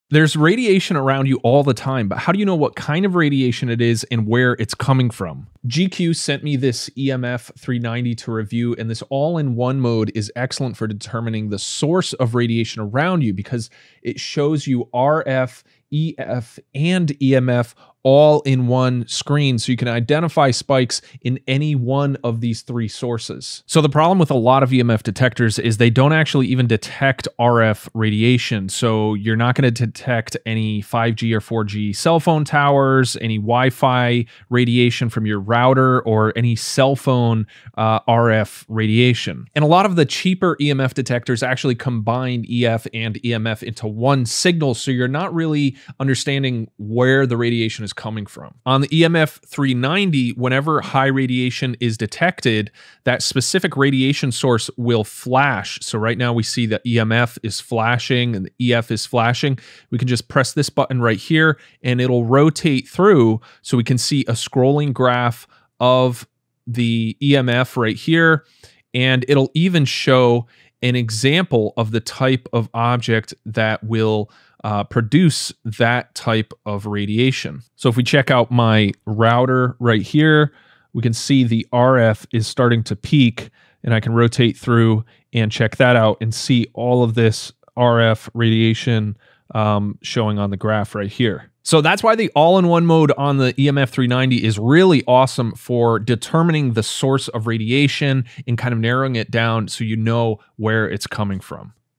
Automatically Identify Radiation Sources with the EMF-390
Welcome to my comprehensive demonstration of the GQ EMF-390, an advanced radiation detector designed for precision and ease of use. In this video, I'm going to explore the capabilities of the GQ EMF-390 in automatically detecting various types of radiation, including RF (Radio Frequency), EF (Electric Field), and EMF (Electromagnetic Field). Whether you're a professional, a hobbyist, or someone concerned about radiation in your environment, this video will show you how easy it is to use the GQ EMF-390 effectively.